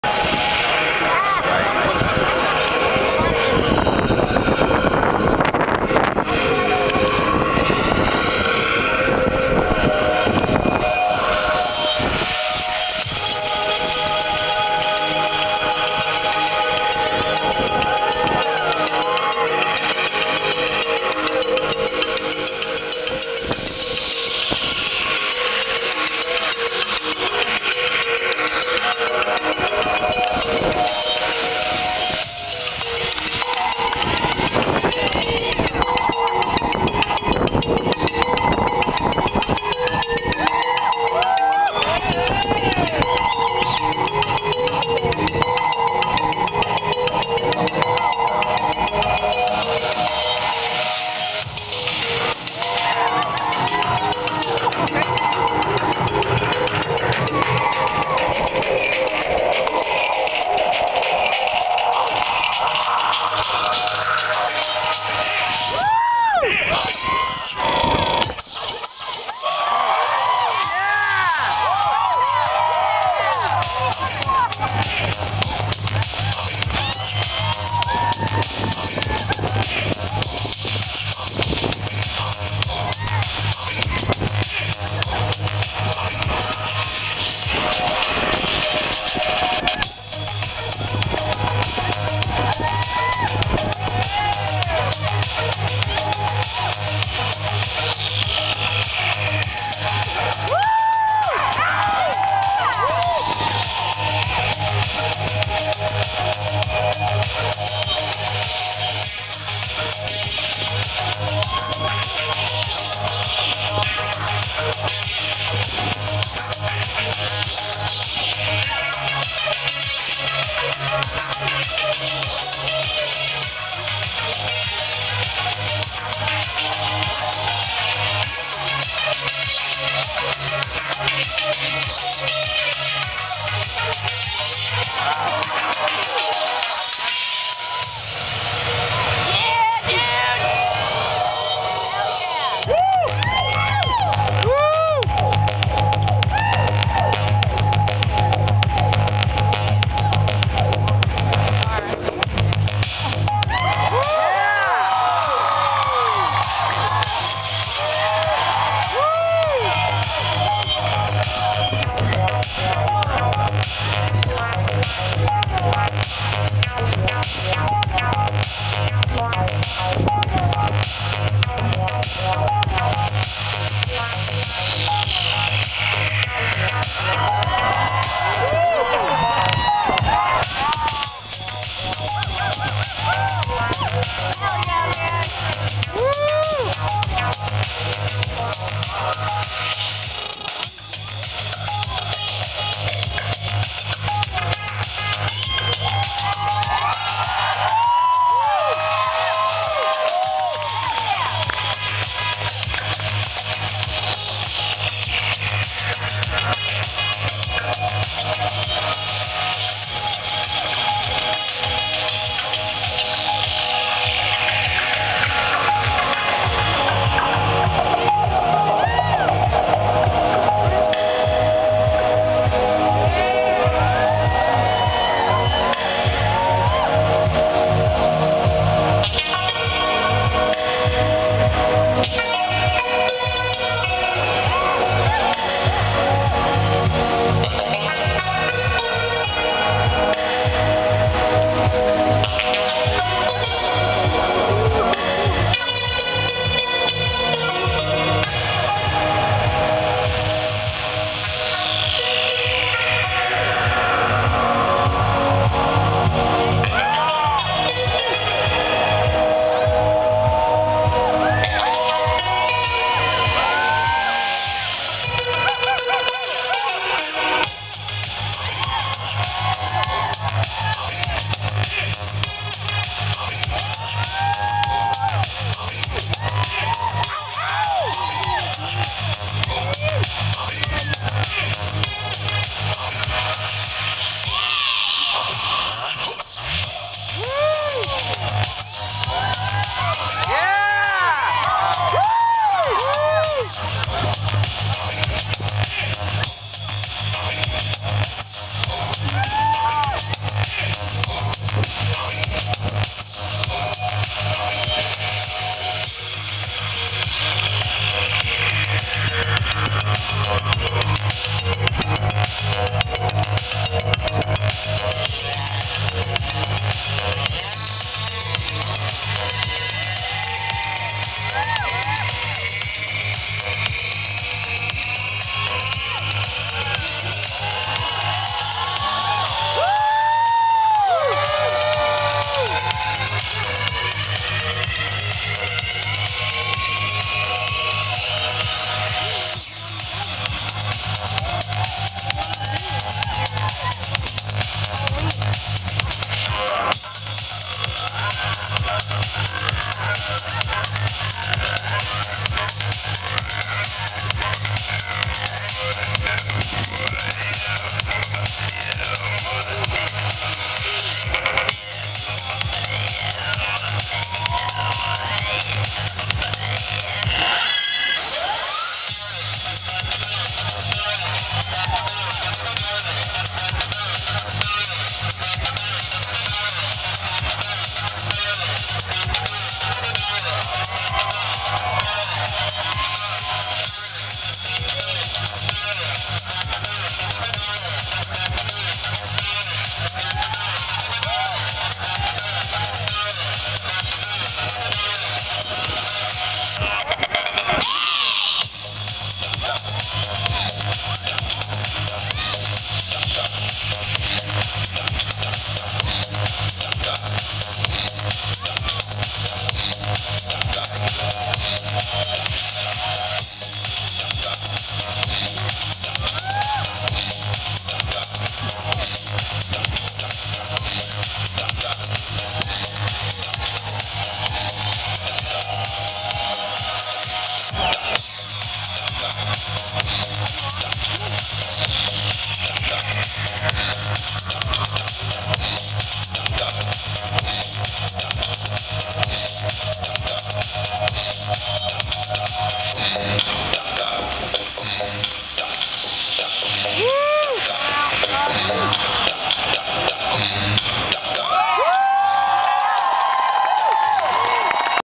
Thomas Johansson of Nevisoul and Alien Jon at the Temple of Poi Fire Expo 2009
Thomas Johansson's and Alien Jon's performances at the Temple of Poi Fire Expo 2009 in Union Square, San Francisco, CA  April 25, 2009.